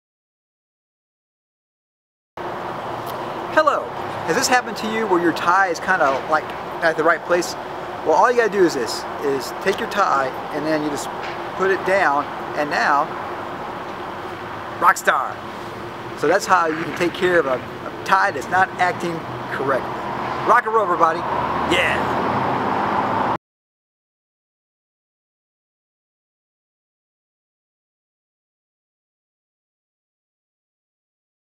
How To Fix Your Tie
Herbert Midgley, the Internet Legend, shows you How To Fix Your Tie!

OBEYYOURROBOTOVERLORDS.COM

THE RISE OF THE ROBOTS on DVD at Amazon Dot Com
THERISEOFTHEROBOTS.NET